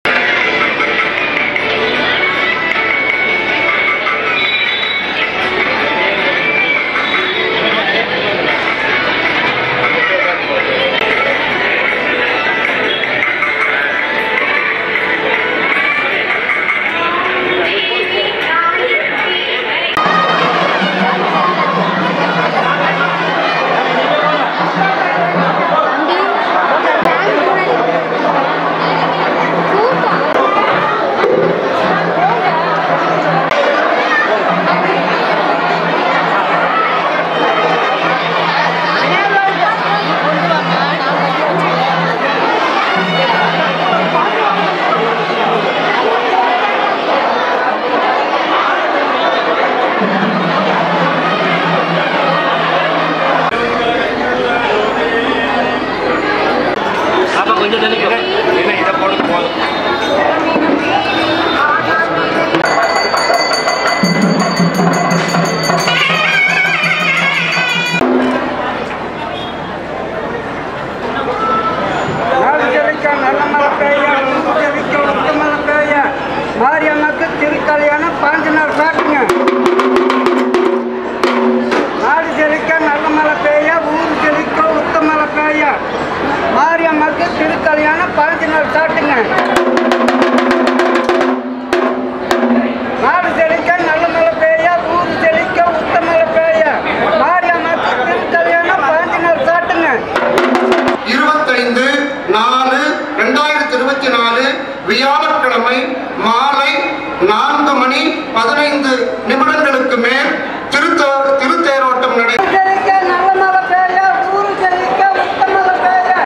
Udumalai mariamman Temple Festival Nonmbu Shorts -Udumalai Mariamman Kovil Thiruvila 2024
உடுமலை மாரியம்மன் கோவில் புதிய தேர் வெள்ளோட்டம்